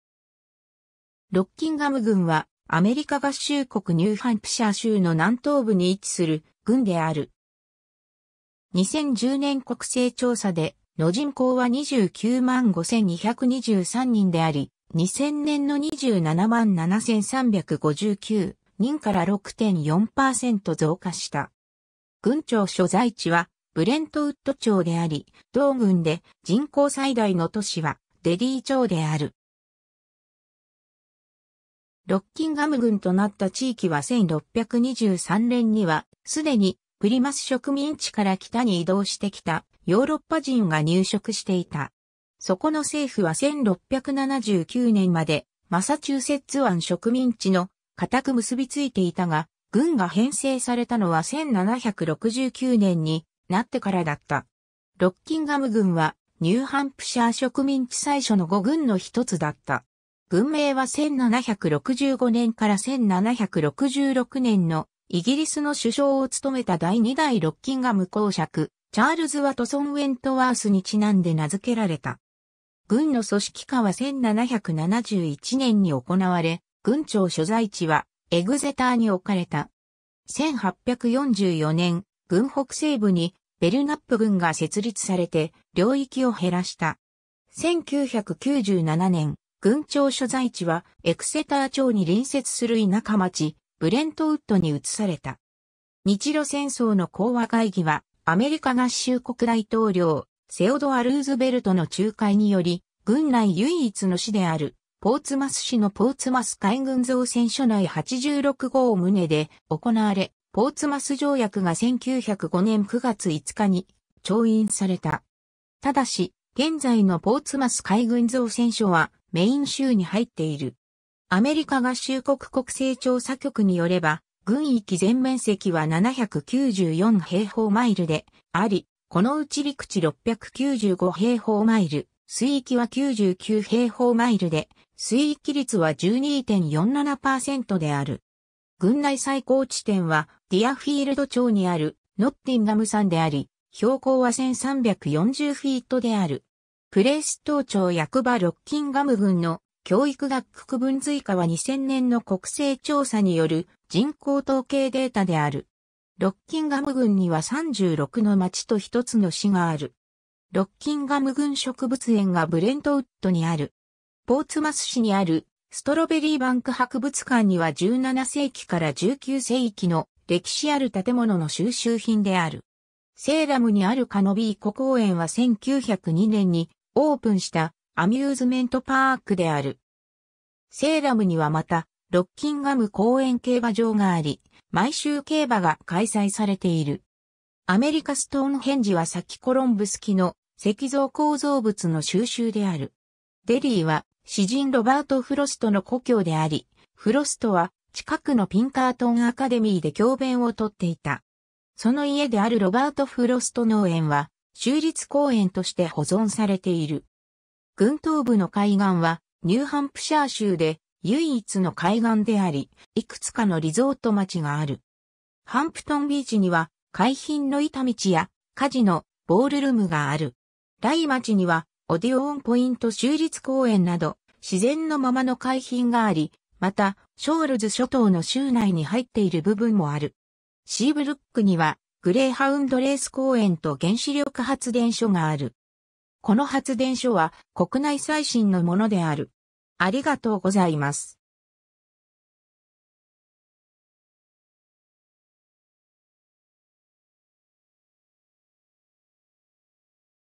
ロッキンガム郡 (ニューハンプシャー州)
ロッキンガム郡（英: Rockingham County）は、アメリカ合衆国ニューハンプシャー州の南東部に位置する郡である。
2010年国勢調査での人口は295,223人であり、2000年の277,359 人から6.4%増加した。
郡庁所在地はブレントウッド町（人口4,486人）であり、同郡で人口最大の都市はデリー町（人口33,109人）である。
 ロッキンガム郡となった地域は1623年には既にプリマス植民地から北に移動してきたヨーロッパ人が入植していた。
そこの政府は1679年までマサチューセッツ湾植民地の堅く結びついていたが、郡が編成されたのは1769年になってからだった。
 ロッキンガム郡はニューハンプシャー植民地最初の5郡の1つだった。
郡名は1765年から1766年のイギリスの首相を務めた第2代ロッキンガム侯爵チャールズ・ワトソン＝ウェントワースに因んで名付けられた。
郡の組織化は1771年に行われ、郡庁所在地はエグゼターに置かれた。
1844年、郡北西部にベルナップ郡が設立されて、領域を減らした。
1997年、郡庁所在地はエクセター町に隣接する田舎町、ブレントウッドに移された。
 日露戦争の講和会議は、アメリカ合衆国大統領セオドア・ルーズベルトの仲介により、郡内唯一の市であるポーツマス市のポーツマス海軍造船所内86号棟で行われ、ポーツマス条約が1905年9月5日に調印された。
ただし、現在のポーツマス海軍造船所はメイン州に入っている。
郡内最高地点はディアフィールド町にあるノッティンガム山であり、標高は1,340フィート (410 m) である。
 プレイストウ町役場 ロッキンガム郡の教育学区区分図 以下は2000年の国勢調査による人口統計データである。
 ロッキンガム郡には36の町と1つの市（ポーツマス市）がある。
 ロッキンガム郡植物園がブレントウッドにある。
ポーツマス市にあるストロベリー・バンク博物館には17世紀から19世紀の歴史ある建物の収集品である。
セイラムにあるカノビー湖公園は1902年にオープンしたアミューズメントパークである。
セイラムにはまた、ロッキンガム公園競馬場があり、毎週競馬が開催されている。
アメリカ・ストーンヘンジは先コロンブス期の石造構造物の収集である。
デリーは詩人ロバート・フロストの故郷であり、フロストは近くのピンカートン・アカデミーで教鞭を執っていた。
その家であるロバート・フロスト農園は州立公園として保存されている。
 郡東部の海岸はニューハンプシャー州で唯一の海岸であり、幾つかのリゾート町がある。
ハンプトンビーチには海浜の板道や火事の・ボールルームがある。
ライ町にはオディオーンポイント州立公園など自然のままの海浜があり、またショールズ諸島の州内に入っている部分もある。
シーブルックにはグレイハウンド・レース公園と原子力発電所がある。
この発電所は国内最新のものである。